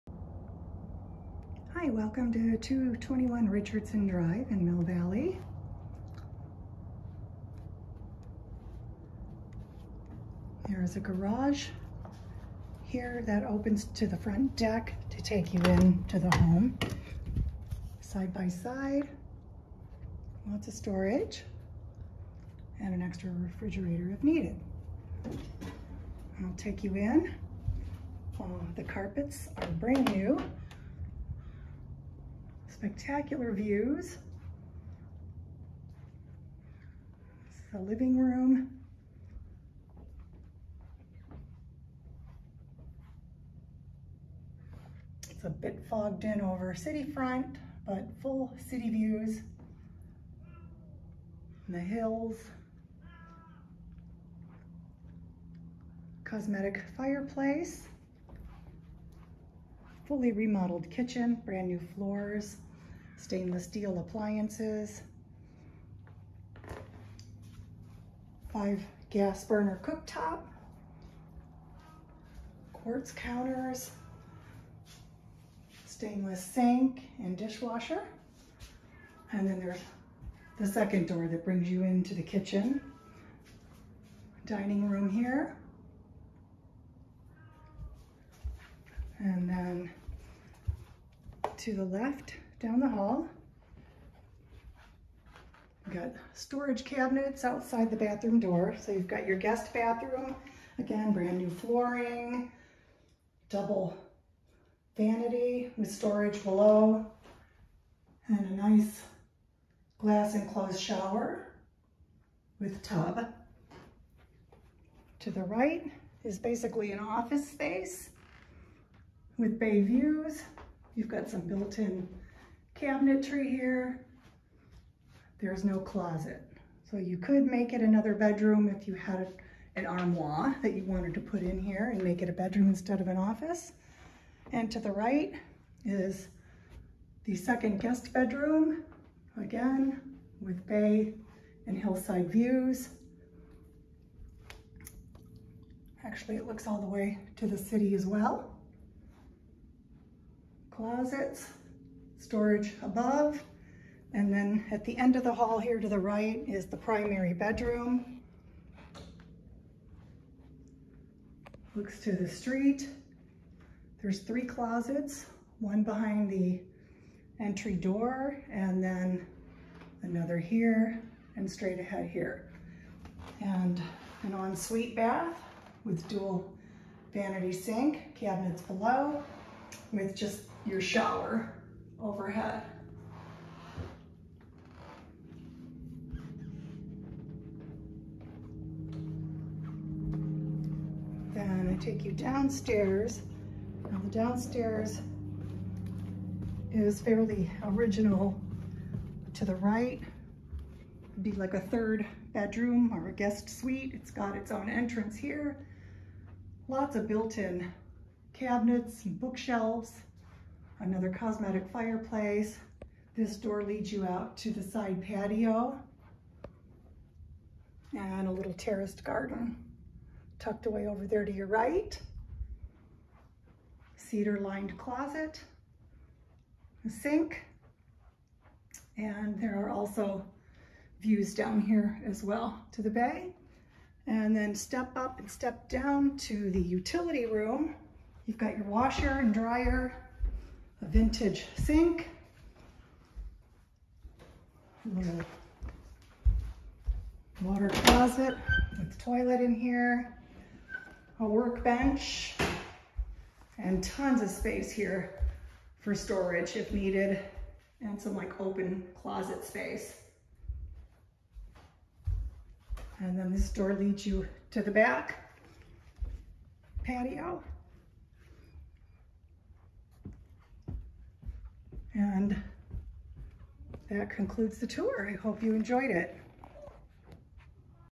221 Richardson Dr. M.V.
Updated 3bd/2ba home, Views of the Bay, S.F. City Front, Bay Bridge, Hills and more! Conveniently located in the Strawberry area of M.V. Quick access to the freeway, shopping centers, dining, and ease of travel to S.F. and Wine Country! You'll enjoy the peace and tranquility this home offers.

The recently remodeled kitchen includes SS KitchenAid appliances, including a 5 gas burner cooktop, built in range, refrigerator and D/W., beautiful quartz counters, tile backsplash, new flooring, and large skylight makes the room light and bright. Drink in the incredible Bay and Hill Views from the comfort of your living/dining room, or relax outdoors on the expansive deck. Cosmetic fireplace in the liv rm.

Both remodeled bathrooms have double vanities with quartz countertops, new lighting, new glass shower doors and new flooring. Master bedroom on main level with en-suite bathroom. Tub/shower combo in bathroom off hallway. Quiet office or den on main floor. All new window treatments and new carpeting in most living areas. Third bedroom on the lower level with Bay View, cedar lined closet, built in shelving and new vanity. Spacious bonus/laundry room with full sized Kenmore W/D, vintage utility sink, additional closets and partial bathroom.

Newly installed drought resistant landscaping completed.”

2 car garage, easy access to 101 freeway, Approx. 10 minutes to the GG Bridge, and 45 min. to the Wine Country. Close to various shopping centers and dining establishments throughout Marin and S.F. Walking distance to Strawberry Recreation Center and Strawberry Pt. elementary school. (There is no pool or spa)

NO PETS PLEASE.

(Assistive animals are always permitted and are approved with legal documentation. No Pet Rent applies for Assistive animals.)